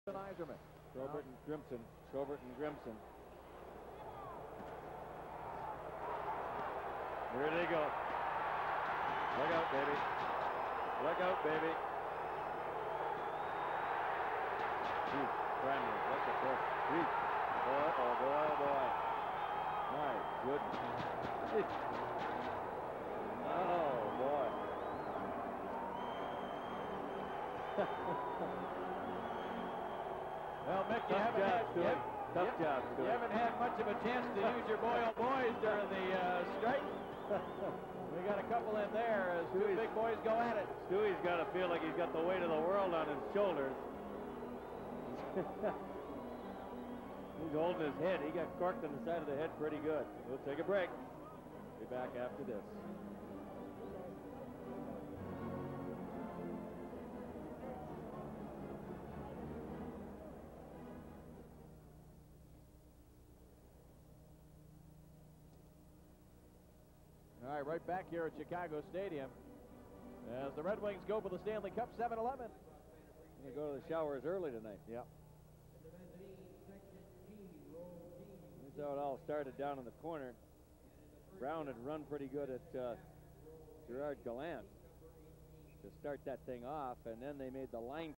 NHL Apr. 12, 1992 Stu Grimson,CHI v Bob Probert,DET Chicago Blackhawks Detroit Red Wings
NHL April 12, 1992 Stu Grimson,CHI v Bob Probert,DET